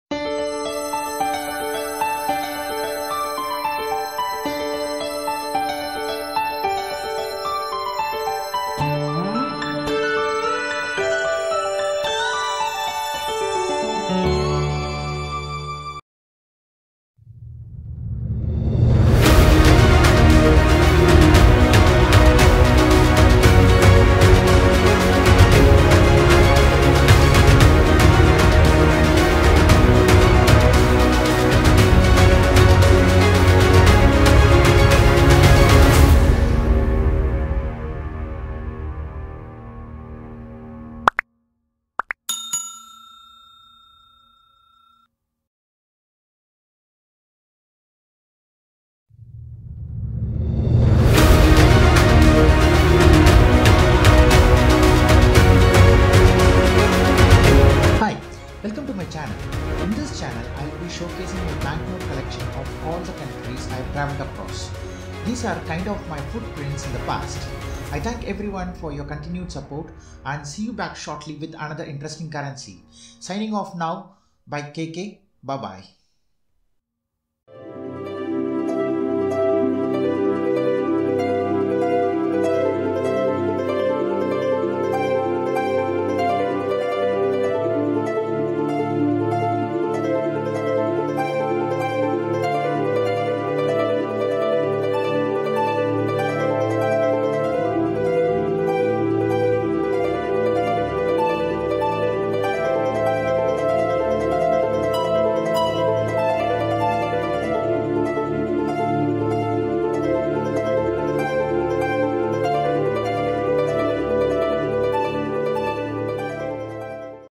RON 1 | Bank Note | One Leu | Kae Kae | CurrencyStackerKK | Romania | Europe
1 Romania Leu

The Romanian leu (Romanian pronunciation: [lew], plural lei [lej]; sign: L; ISO 4217 code: RON; numeric code: 946) is the currency of Romania. It is subdivided into 100 bani (Romanian pronunciation: [banʲ], singular: ban [ban]), a word that means "money" in Romanian.

The name of the currency means "lion", and is derived from the Dutch thaler (leeuwendaalder "lion thaler/dollar"). The Dutch leeuwendaalder was imitated in several German and Italian cities. These coins circulated in Romania, Moldova and Bulgaria and gave their name to the respective currencies the Romanian leu, Moldovan leu, and Bulgarian lev.

First Leu [1867–1947] - In 1860, the Domnitor Alexandru Ioan Cuza attempted to create a national românul ("the Romanian") and the romanat; however, the project was not approved by the Ottoman Empire. On 22 April 1867, a bimetallic currency was adopted, with the leu equal to 5 grams of 83.5% silver or 0.29032 grams of gold. The first leu coin was minted in Romania in 1870.

Second Leu [1947–1952] -  A revaluation ("Great stabilization", marea stabilizare) took place on 15 August 1947, replacing the old leu at a rate of 20,000 old lei = 1 new leu.

Third Leu ROL [1952–2005] - On 28 January 1952, another new leu was introduced. Unlike the previous revaluation, different rates were employed for different kinds of exchange (cash, bank deposits, debts etc.) and different amounts. These rates ranged from 20 to 400 "old lei" for one "new" leu. Again, no advance warning was given before the reform took place.

Fourth Leu RON [2005–present] - On 1 July 2005, the leu was revalued at the rate of 10,000 "old" lei (ROL) for one "new" leu (RON), thus psychologically bringing the purchasing power of the leu back in line with those of other major Western currencies. The term chosen for the action was "denominare", similar to the English term "redenomination".

KokulaKrishnaHariK - ASDF International Secretary - Computer Science Specialist. for more information.